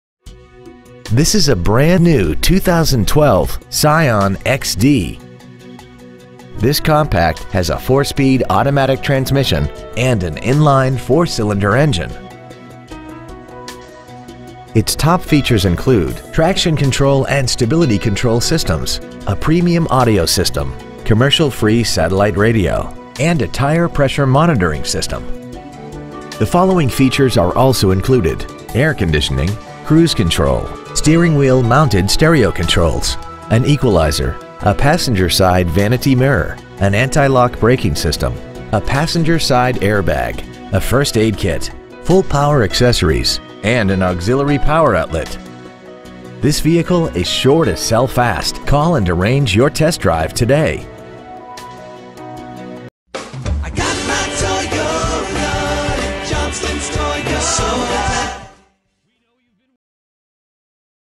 2012 Scion xD New Hampton NY 10958
Year : 2012
 Make : Scion
 Model : xD
 Engine : Gas I4 1.8L/110
 Trans . : Automatic
 Exterior : Amazon Green Metallic
 Miles : 2
 Interior : Dark Charcoal
 Stock : 121856